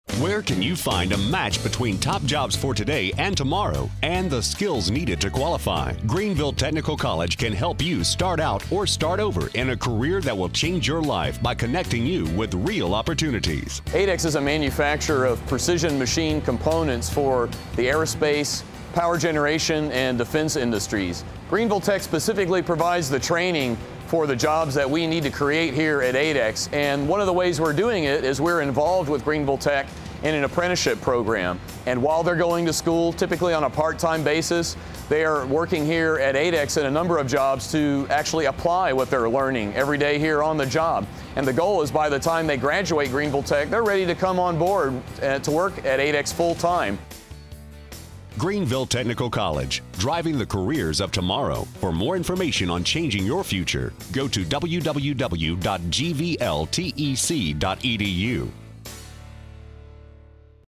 ADEX CEO talks about Greenville Tech apprenticeship program
Jason Premo, ADEX Machining Technologies, LLC
Machine Tool Technology
Driving the Careers of Tomorrow TV spot for Greenville Technical College